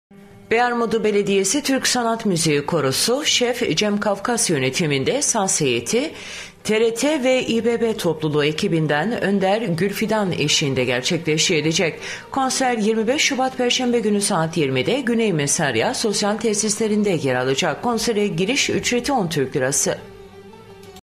BEYARMUDU BELEDİYESİ TÜRK SANAT MÜZİĞİ KONSERİ
Beyarmudu Belediyesi Türk Sanat Müziği Korusu, Şef Cem Kafkas yönetiminde Saz heyeti TRT ve İBB topluluğu ekibinden Önder Gülfidan eşliğinde gerçekleştirilecek.
Konser 25 Şubat perşembe günü saat 20.00’ de Güney Mesarya Sosyal Tesislerinde yer alacak. Konsere giriş ücreti 10 TL. dir.